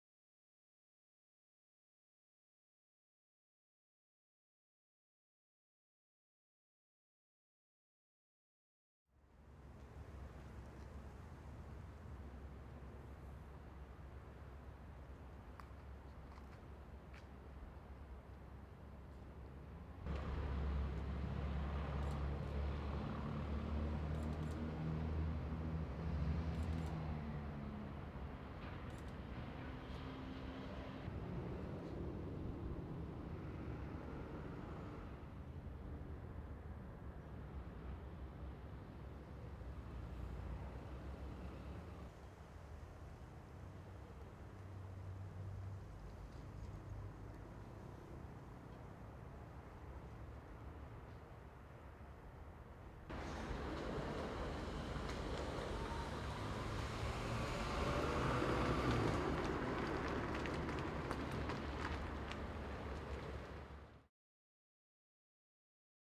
AJA KiPRO mini
VerUPしたKipromini、HDMIケーブルでTC・トリガーを含むコントロール信号が引き継げるようになりました。なのでせっかくトリガーで同期できるので、FS100標準コーデックのAVCHDとKiproコーデックのProres422（LT)での比較をしてみました。youtube圧縮で少々解りづらいですが暗部での階調の良さが出ています。